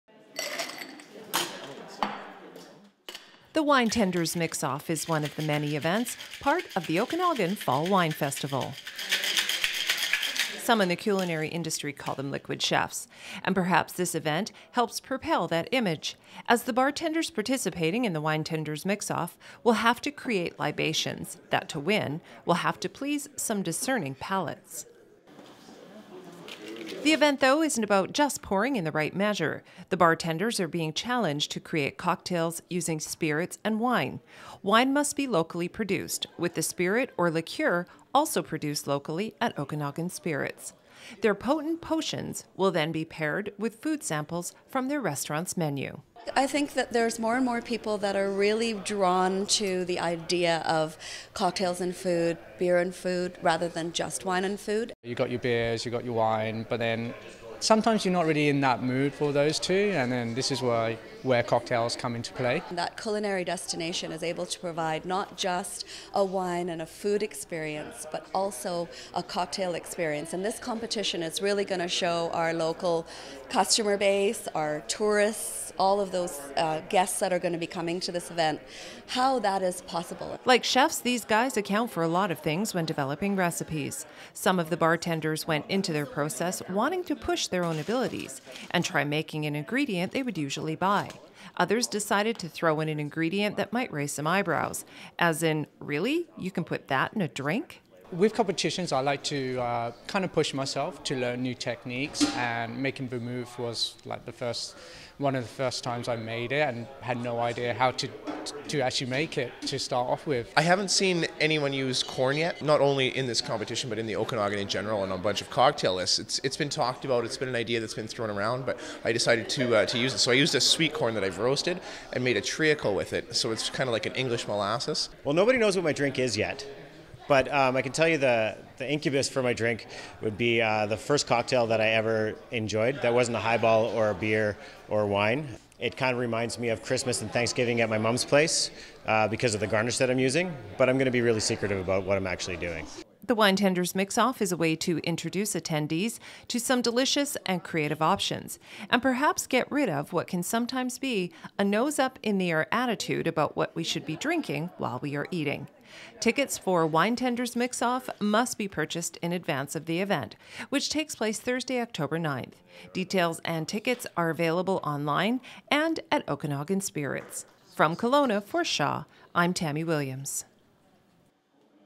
Winetender
A preview of one of the many events coming up as part of the Okanagan Fall Wine Festival.